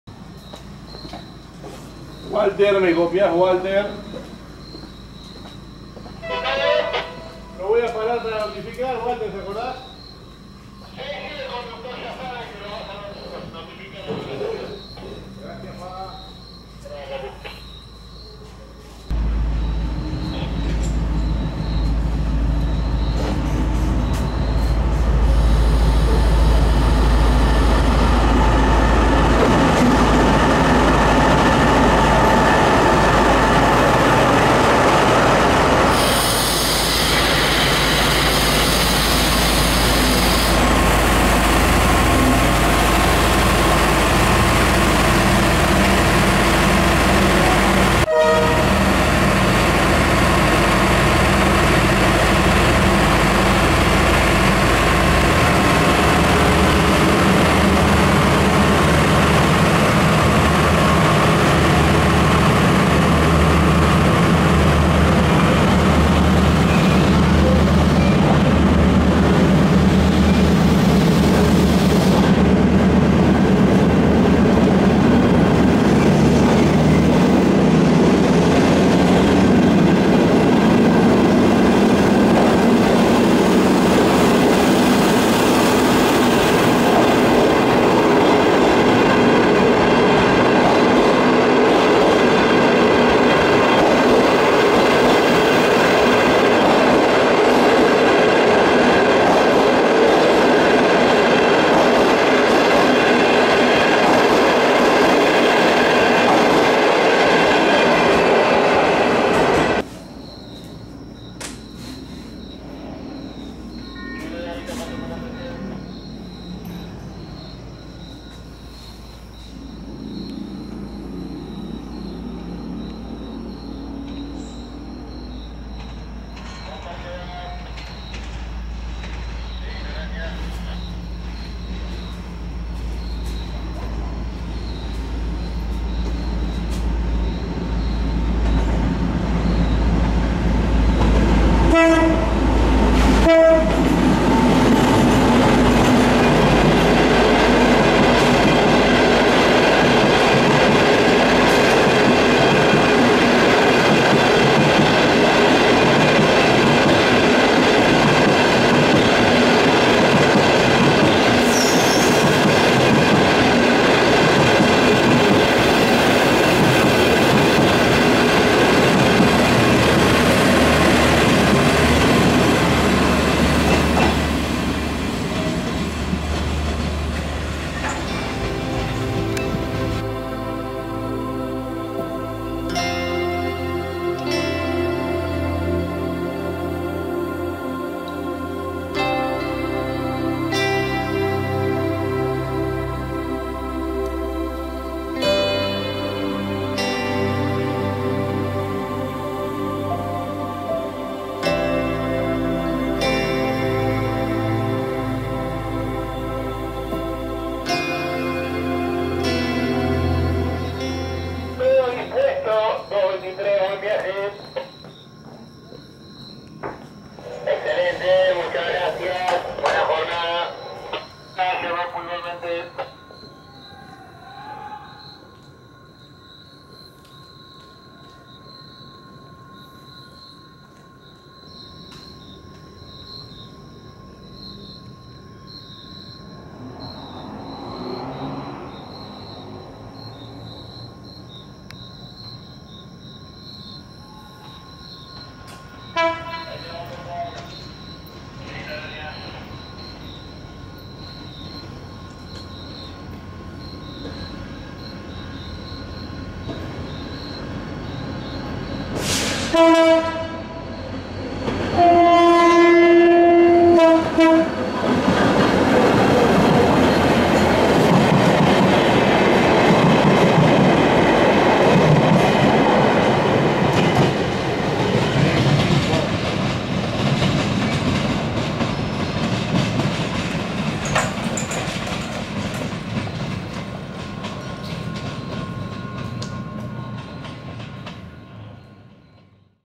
Así es el Movimiento Nocturno de Trenes en BRANDSEN | Tren a Mar del Plata | Trains 2023
Buenas buenas para todos! Un ritual con comunicaciones y pasadas en la madrugada de un pueblo. Los trenes es lo único que se escucha después de las 00hs y aquí intentamos retratarlos por la estación Brandsen. Espero que disfruten este video, mi voz no aparece pero si la voz de la radio ferroviaria. Saludos para todos!! @fedemop